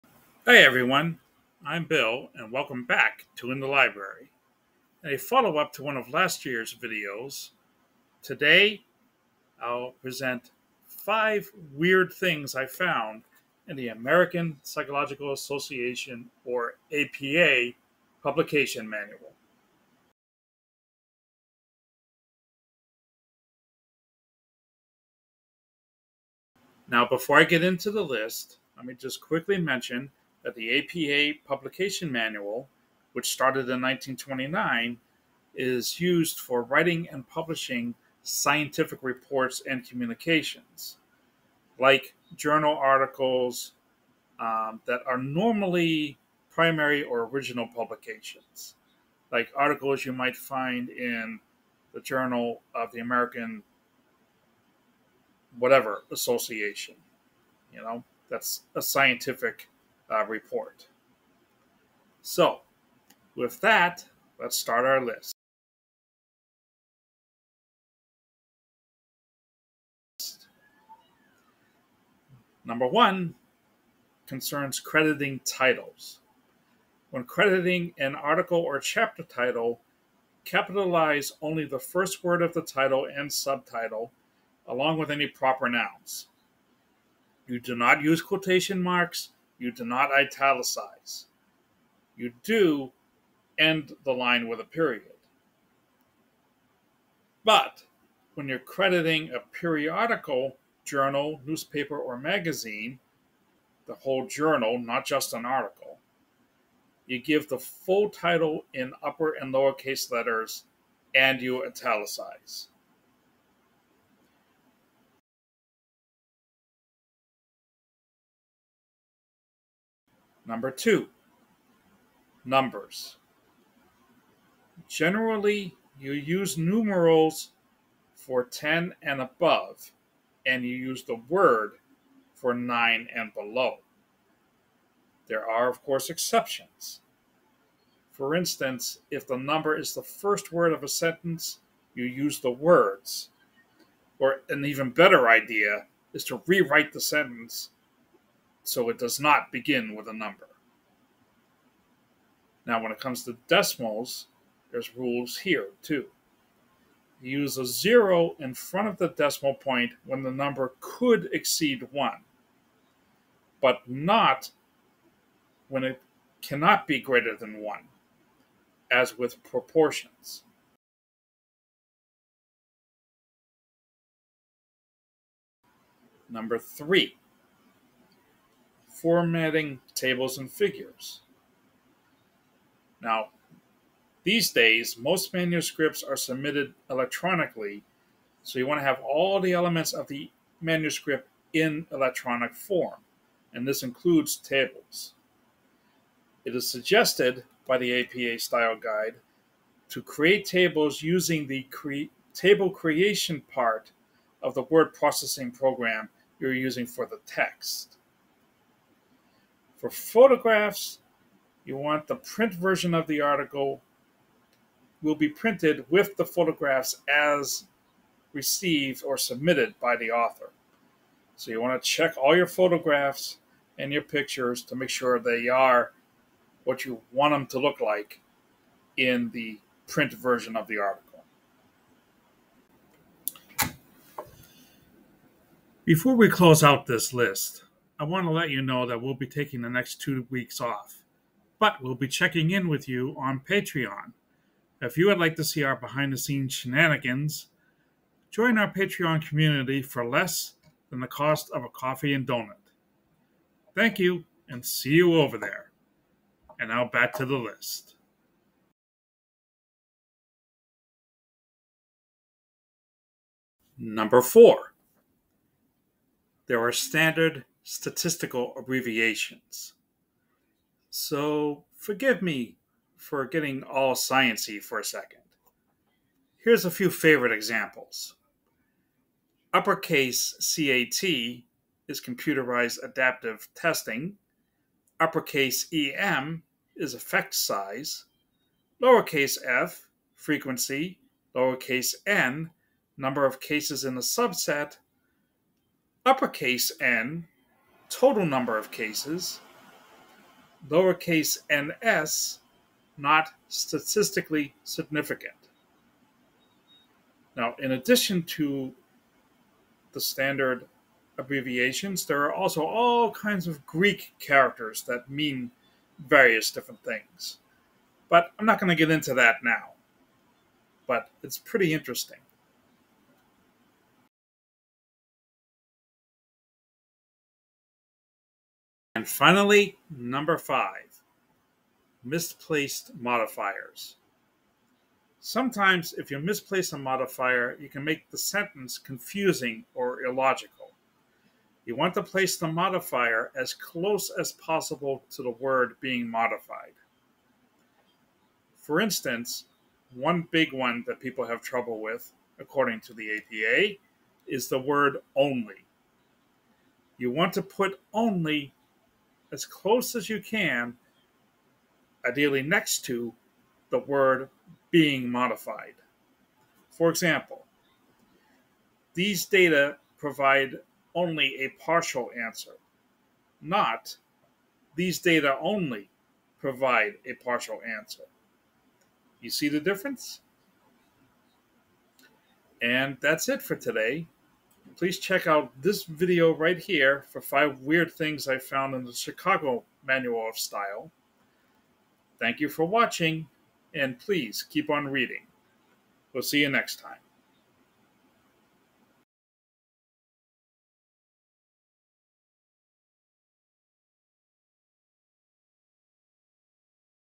5 Weird Things I found in the APA Style Guide
This week, I take a look at the APA Style Guide, which was first published in 1929.

Here are 5 of the weird things that that I found.

Also, the stupendous (albeit brief) return of Editor Bill!

My Handle: @inthelibraryBG